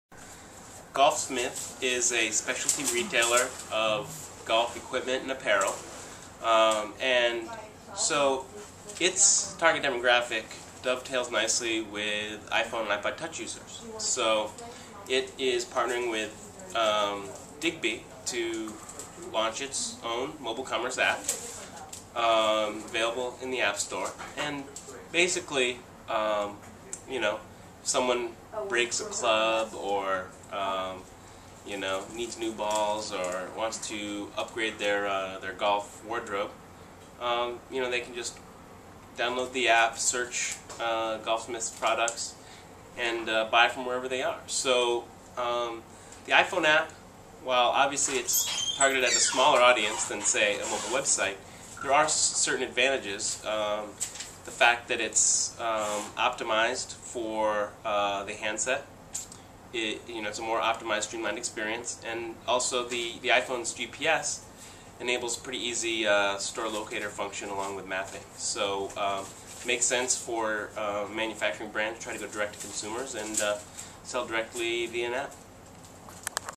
Buy golf equipment and apparel using your iPhone or iPod touch
Golfsmith is hoping that its Digby-powered commerce app is a hole in one.